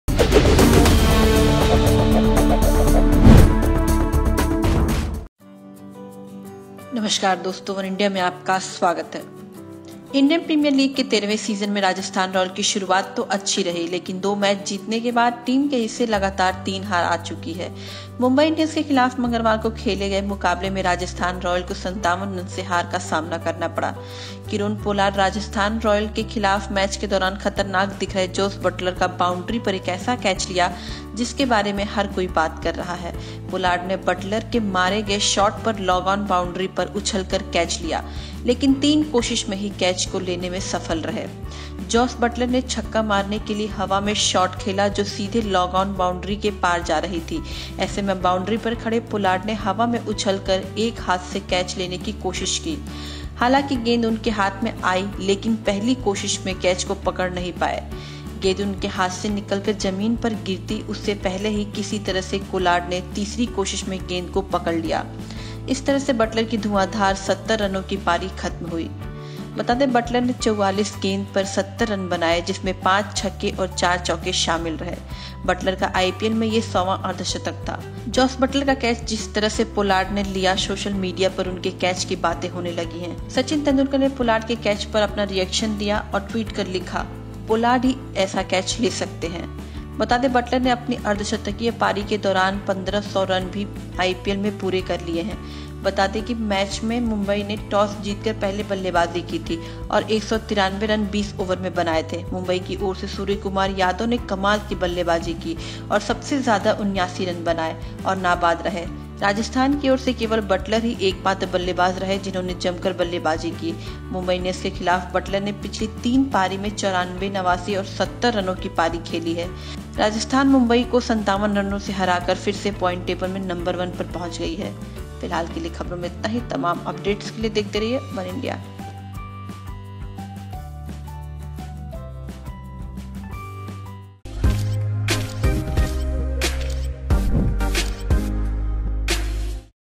IPL 2020: Sachin Tendulkar praises Pollard's freak catch to dismiss Jos Buttler | वनइंडिया हिंदी
During the 20th match of the ongoing 13th season of the Indian Premier League between Mumbai Indians and Rajasthan Royals in Abu Dhabi Mumbai Indians all-rounder Kieron Pollard grabbed a freak catch at a crucial juncture in the match to dismiss Rajasthan Royals wicket-keeper batsman Jos Buttler.

इंडियन प्रीमियर लीग के 13वें सीजन में राजस्थान रॉयल्स की शुरुआत तो अच्छी रही, लेकिन दो मैच जीतने के बाद टीम के हिस्सा लगातार तीन हार आ चुकी हैं. मुंबई इंडियंस के खिलाफ मंगलवार को खेले गए मुकाबले में राजस्थान रॉयल्स को 57 रन से हार का सामना करना पड़ा. किरॉन पोलार्ड ने राजस्थान रॉयल्स के खिलाफ मैच के दौरान खतरनाक दिख रहे जोस बटलर का बाउंड्री पर एक ऐसा कैच लिया जिसके बारे में हर कोई बात कर रहा है.